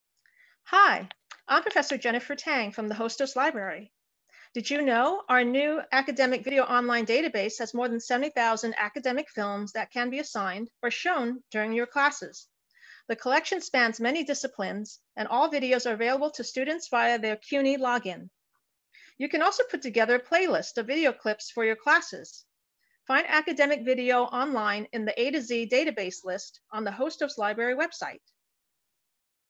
Did You Know? AVON Database
Introduces the Academic Video Online database, which has more than 70,000 academic films that can be assigned or shown during Hostos classes. The collection spans many disciplines and all videos are available to students' via their CUNY Login. You can also put together a playlist of video clips for your classes. Find Academic Video Online in the A-Z Database list on the Hostos Library website.